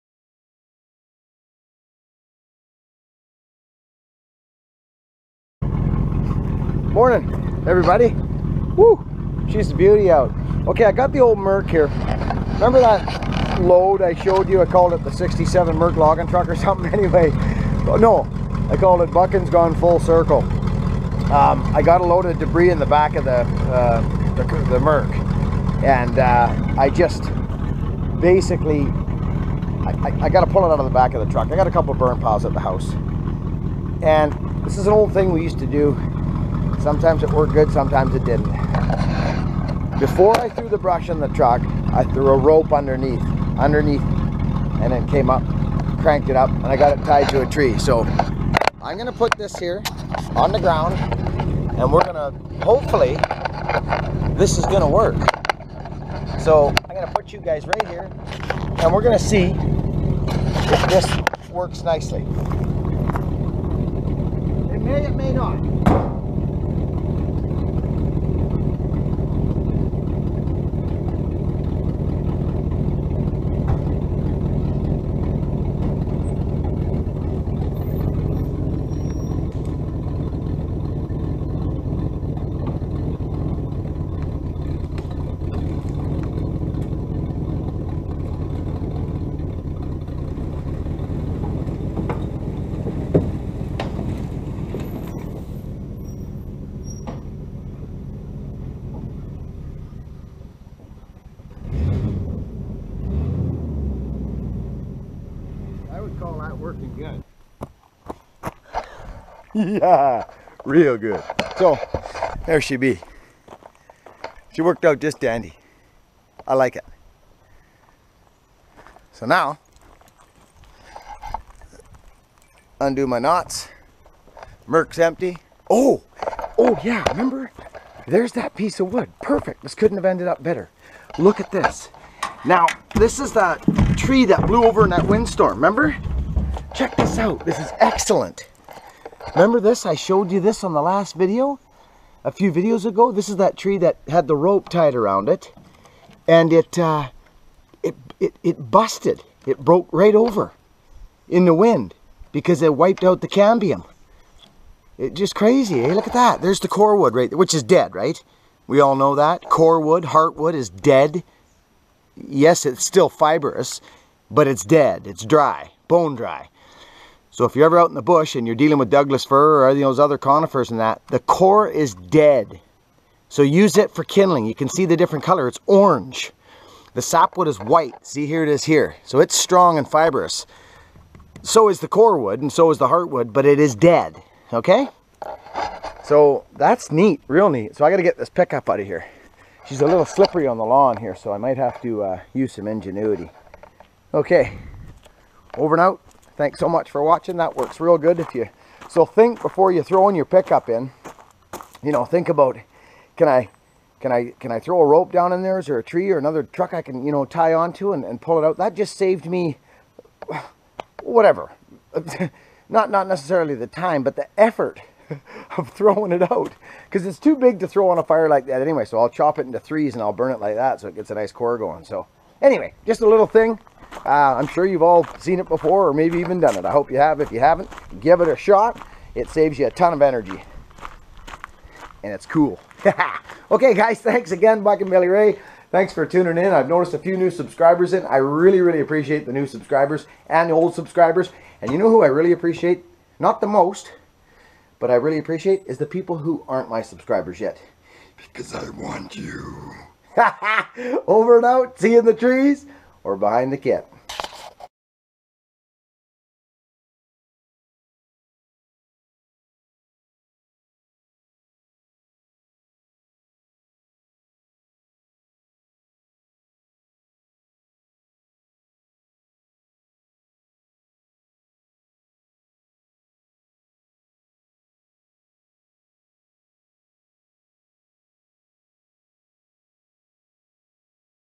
REMOVAL OF BRANCHES FROM TRUCK BOX
Description     WE USE TO DO THIS LOTS BEFOR THE DAYS OF CHIPPERS, ALEAST BEFOR I COULD AFFOARRD ONE